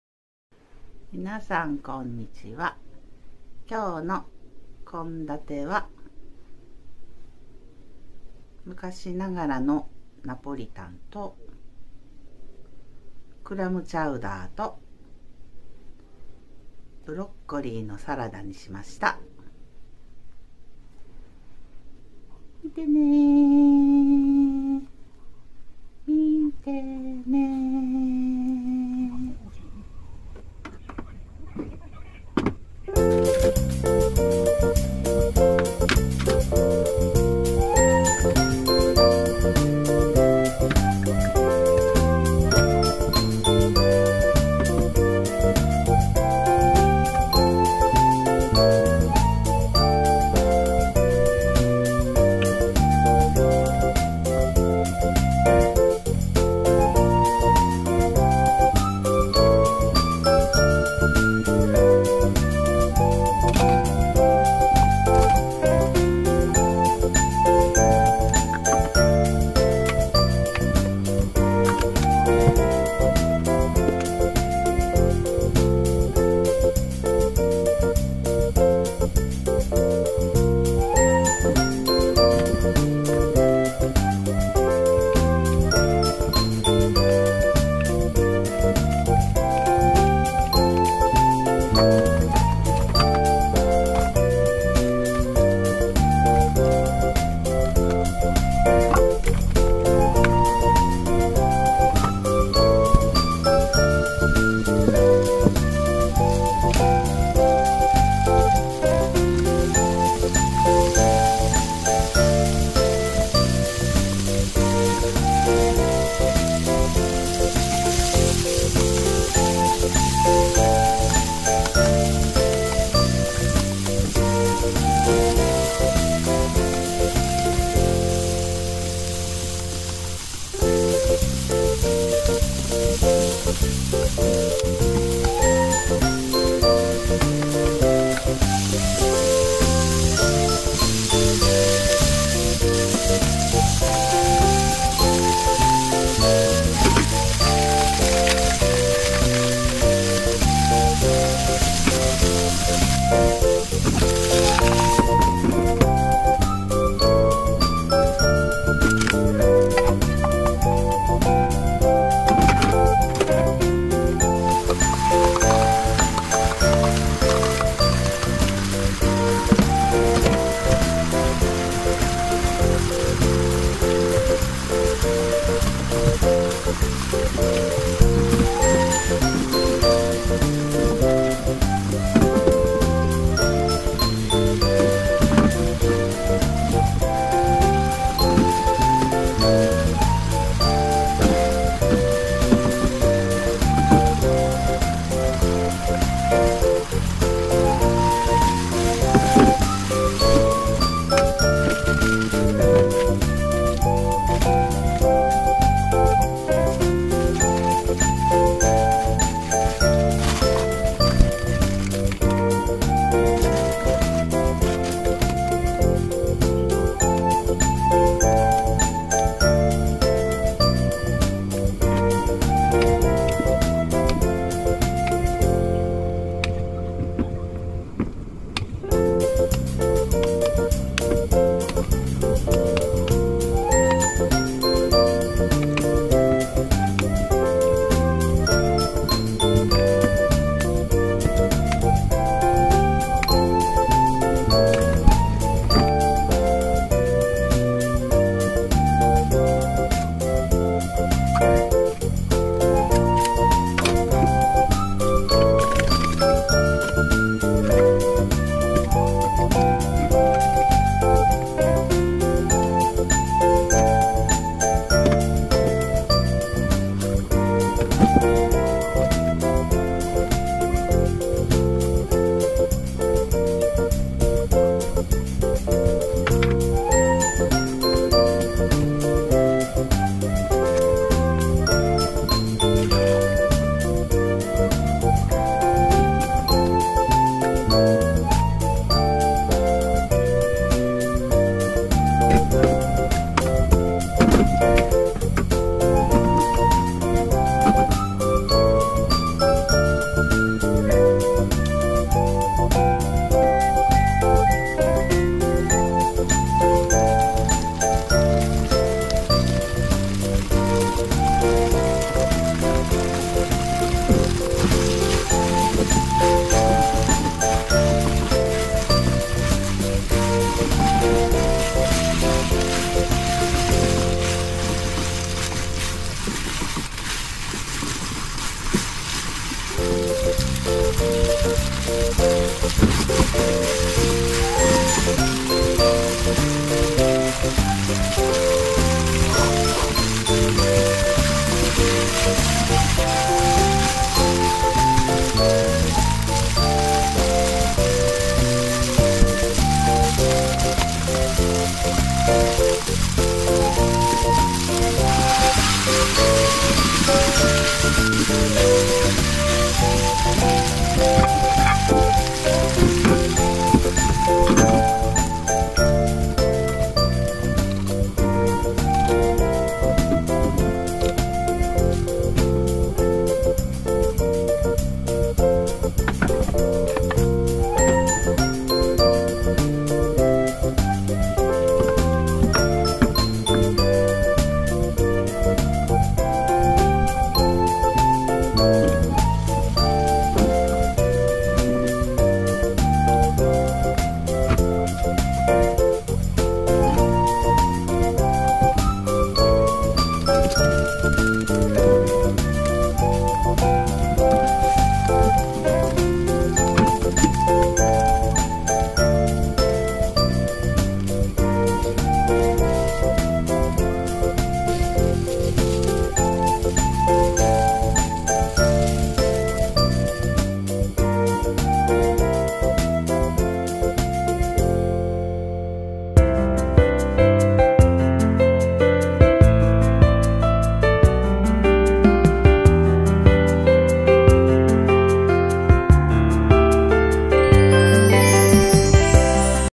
献立に困ったら、昔ながらのナポリタンとクラムチャウダー、ブロッコリーのサラダで🤭
ご視聴有難うございます。今日は完全に手抜きです🤣でも凄く美味しかったのでぜひ献立に困った時、作ってみて下さい🤭